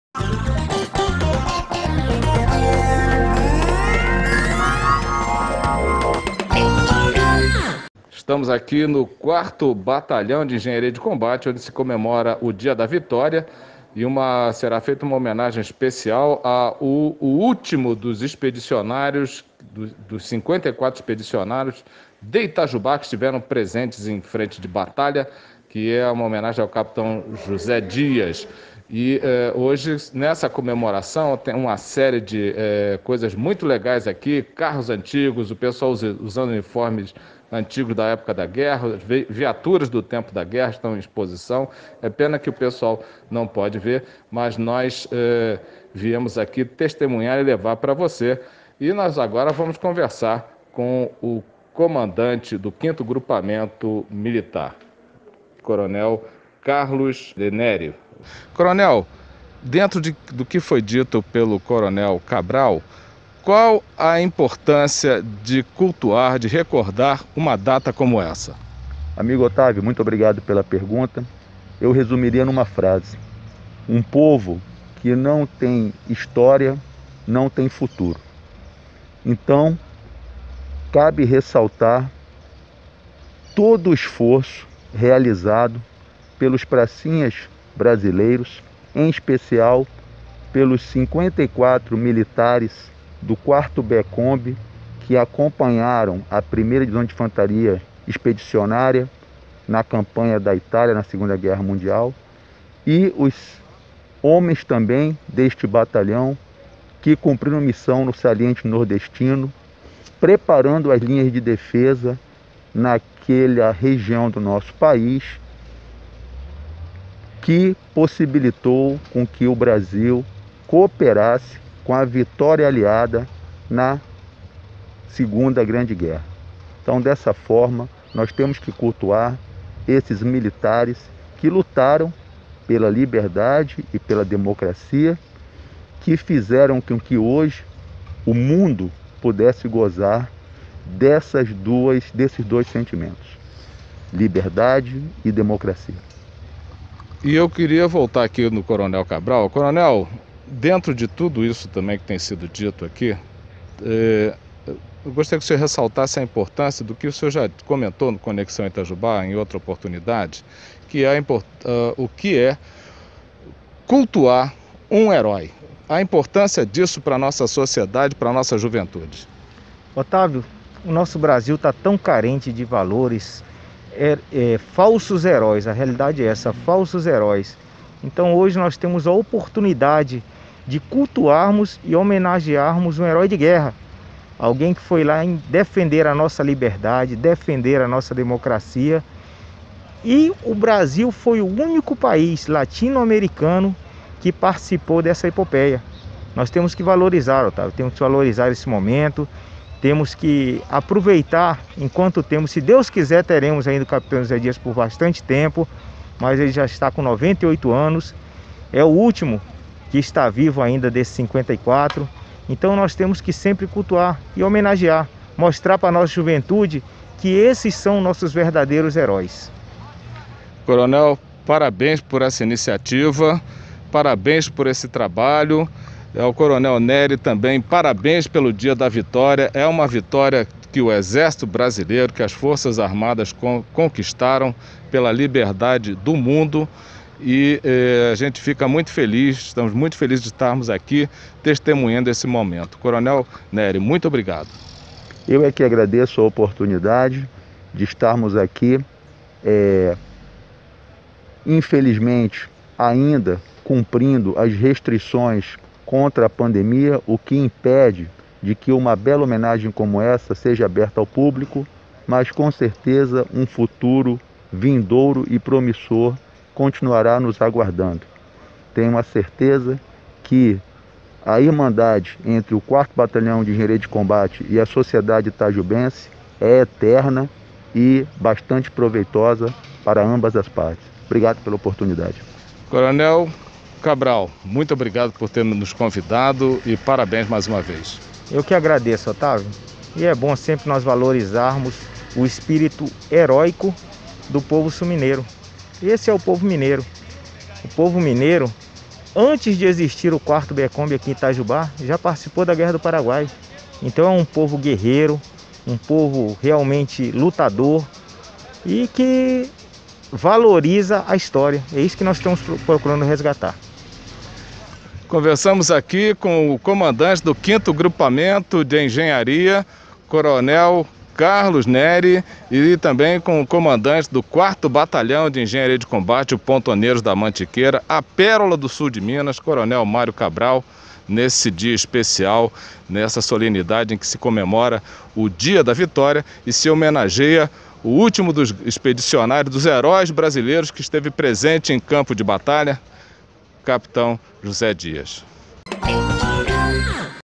4º B E Comb comemora o Dia da Vitória
O evento contou com a presença do comandante da 4ª Região Militar o Gen Div Jorge Antonio Smicelato e foi homenageado o último dos 54 expedicionários itajubenses que estiveram em combate, o Cap. José Dias, hoje com 98 anos.